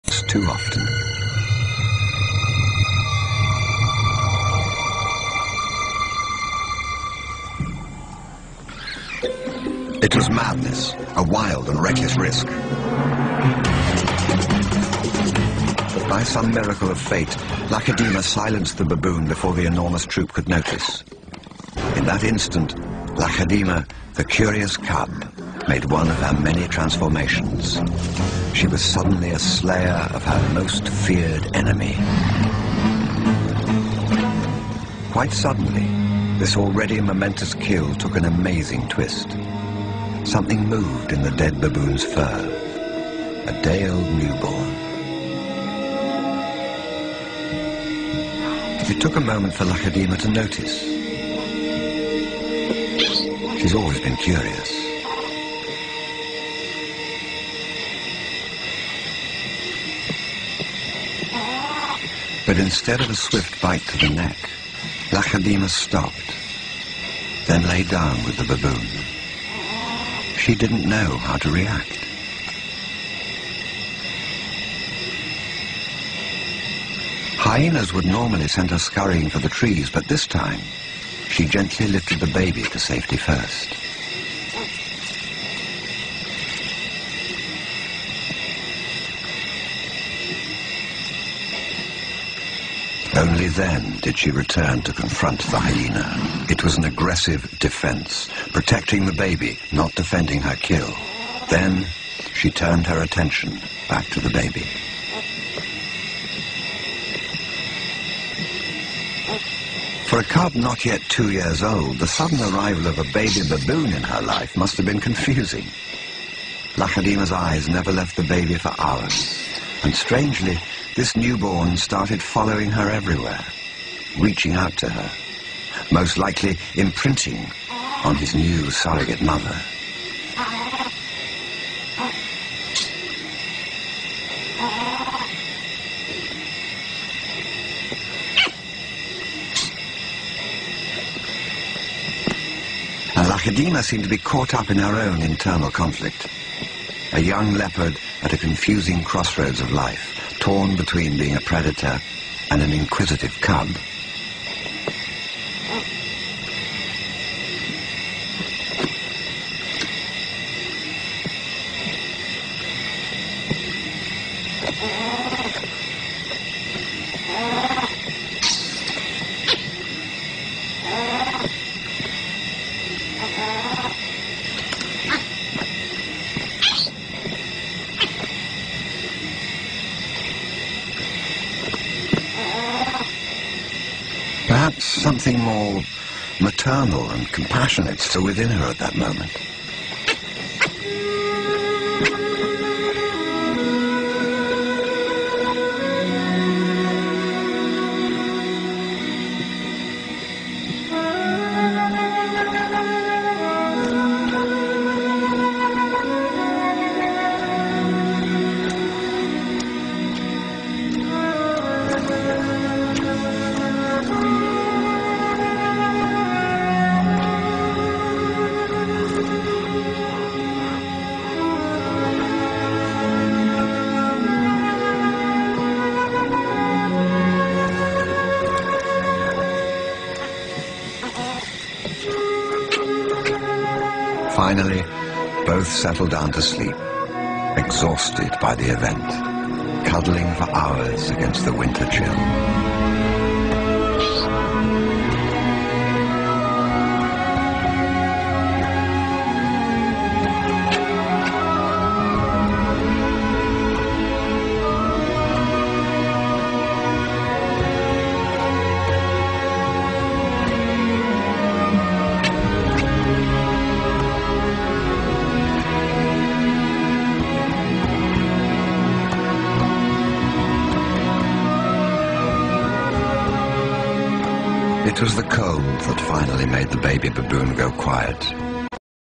amazing wildlife video
Leopard cuddles Baby Baboon